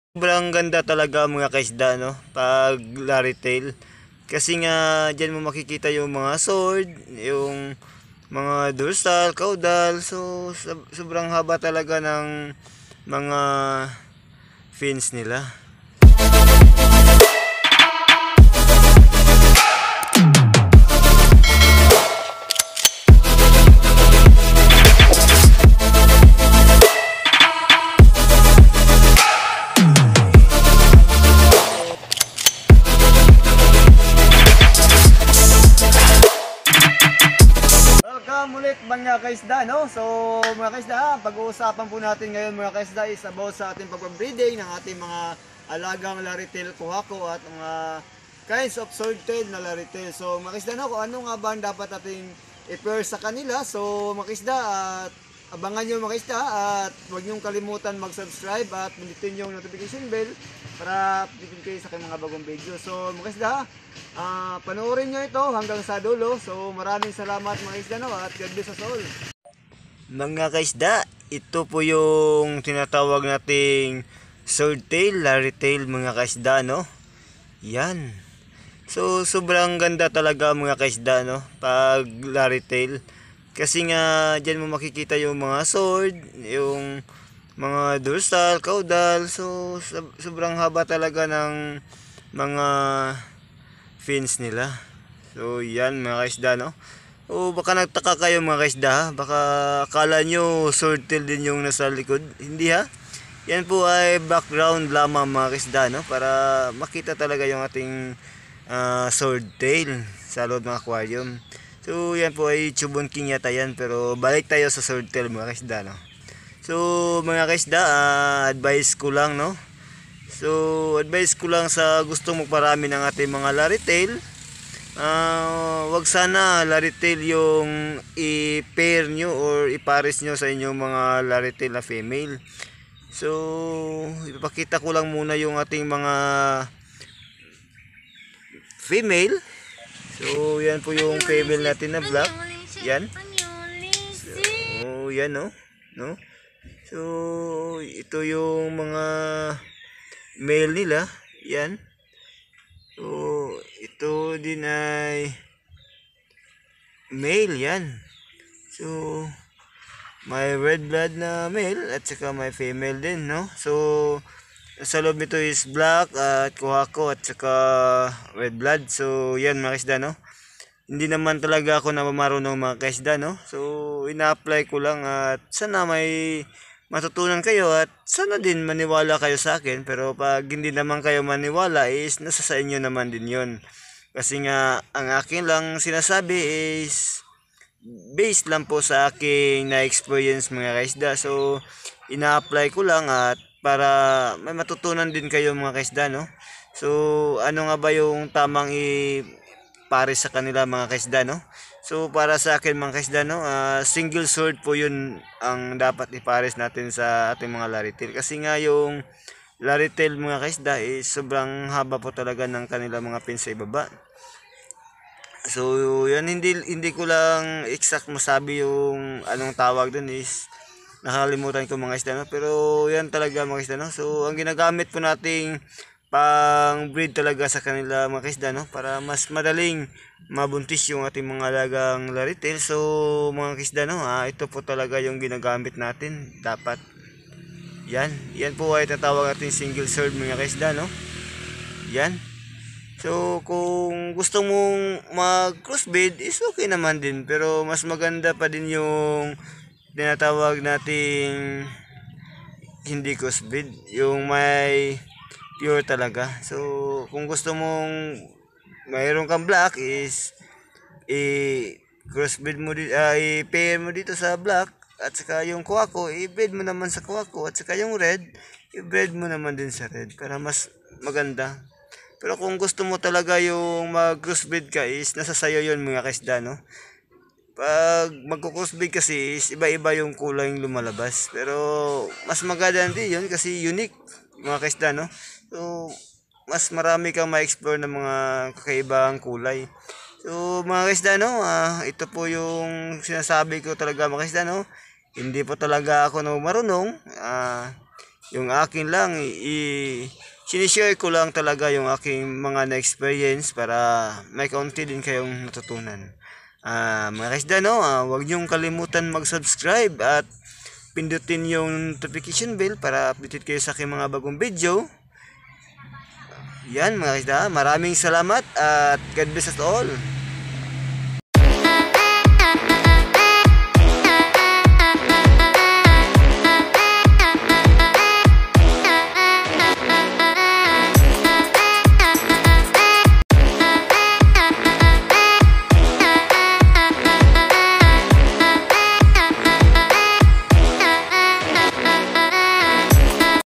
Swordtail lairytail Breed to Single Sword ,(The secret of breeding)
Tips para mabilis mabuntis at maparami  ating mga alagang Swordtail...
Lairytail Swordtail breed to Single Sword.